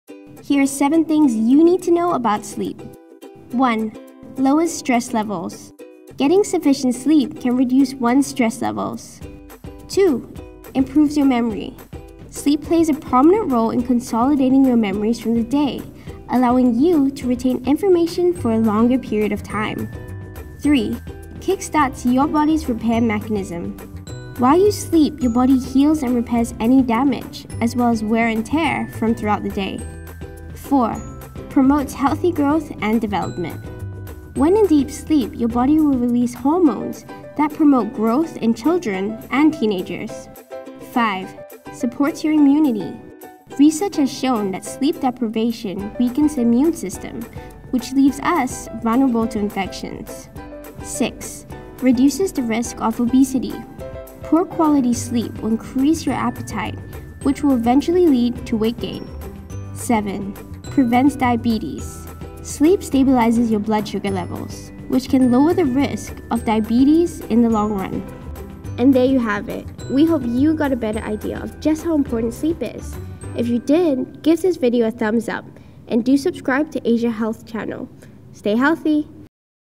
7 Things You Need To Know About Sleep
Discover how these 7 facts about sleep can affect your health!